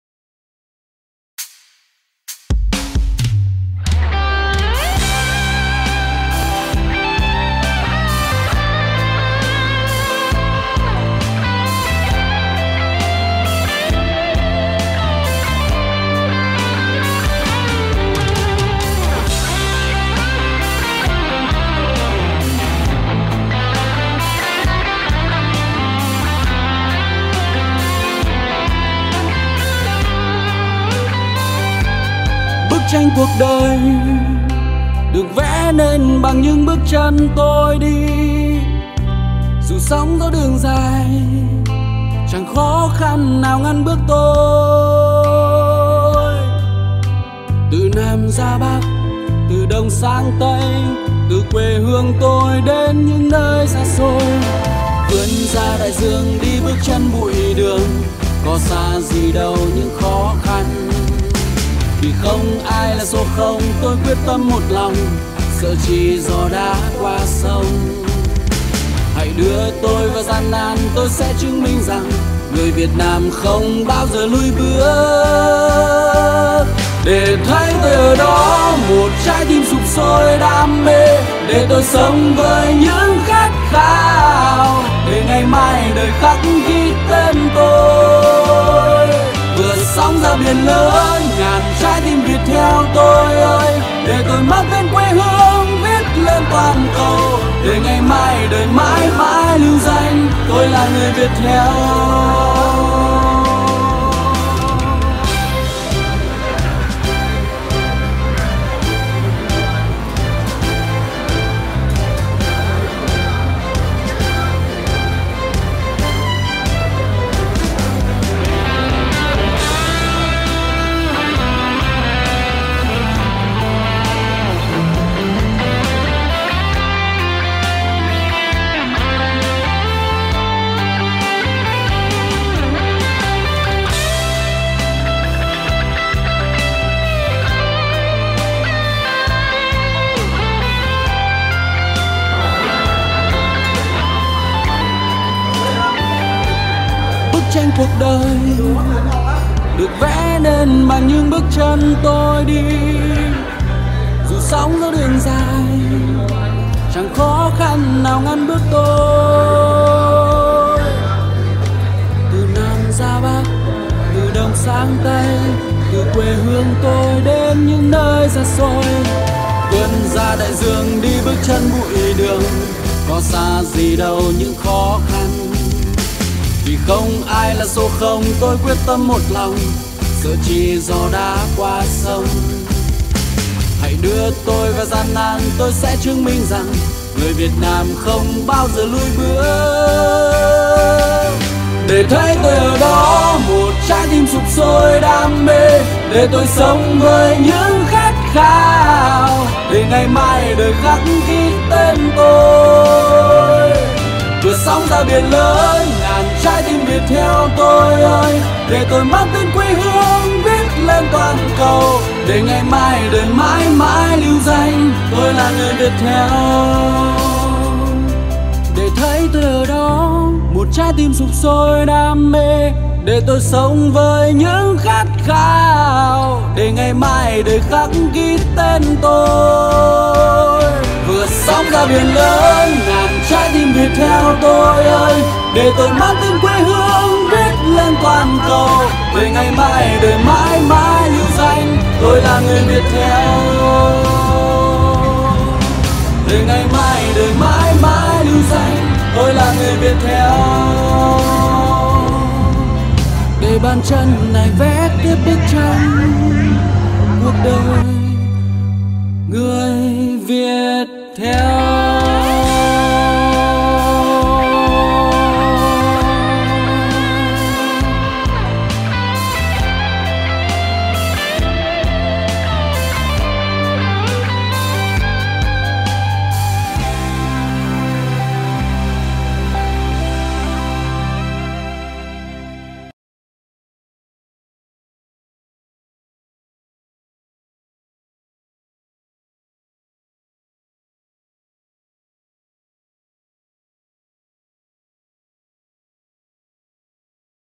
Khóa đào tạo Kỹ năng xây dựng nội dung &tìm kiếm khách hàng trên các nền tảng số - Viettel Quảng Nam
Khóa đào tạo Kỹ năng xây dựng nội dung &tìm kiếm khách hàng trên các nền tảng số - Viettel Quảng Nam - Công ty LCT - Giảng viên Nguyễn Phan Anh
Trụ sở: 3A/19, 59 Lê Đức Thọ, Nam Từ Liêm, HN